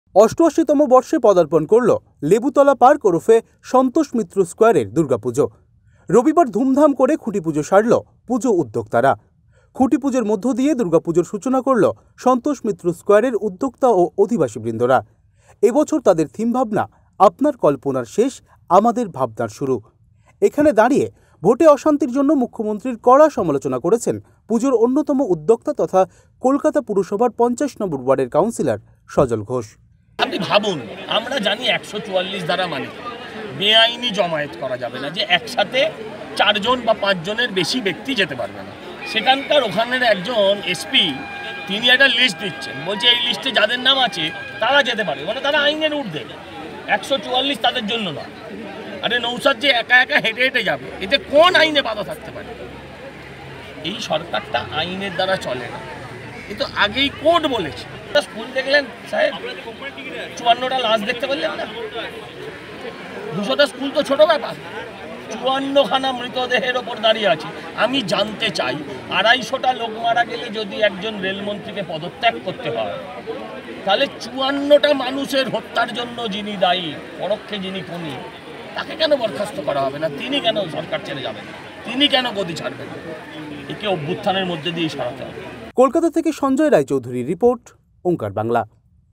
ভোট অশান্তি নিয়ে মুখ্যমন্ত্রীকে কড়া সমালোচনা সজলের
৮৮ তম বর্ষে পদার্পণ করল লেবুতলা পার্ক ওরফে সন্তোষ মিত্র স্কোয়ারের দুর্গাপুজো। রবিবার ধুমধাম করে খুঁটি পুজো  সারল পুজো উদ্যোক্তারা। খুঁটি পুজোর মধ্যে দিয়ে  দুর্গাপুজোর সূচনা করল সন্তোষ মিত্র স্কোয়ারের উদ্যোক্তা ও অধিবাসীবৃন্দরা।এবছর তাদের থিম ভাবনা আপনার কল্পনার শেষ আমাদের ভাবনার শুরু। এখানে দাঁড়িয়ে ভোটে অশান্তির জন্য মুখ্যমন্ত্রীর কড়া সমালোচনা করেছেন পুজার অন্যতম উদ্যোক্তা তথা কলকাতা পুরসভার ৫০ নম্বর ওয়ার্ডের কাউন্সিলর  সজল ঘোষ।